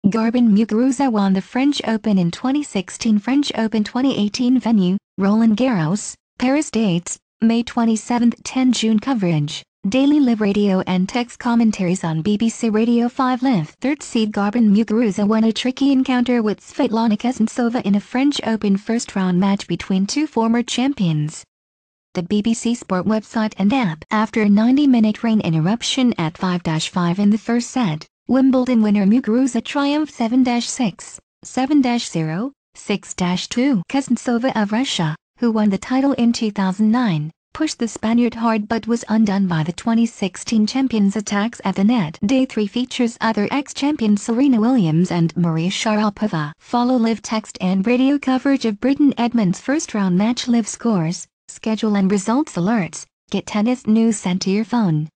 [2018 NEWS]French Open 2018: Garbine Muguruza beats Svetlana Kuznetsova in first round
French Open 2018: Garbine Muguruza beats Svetlana Kuznetsova in first round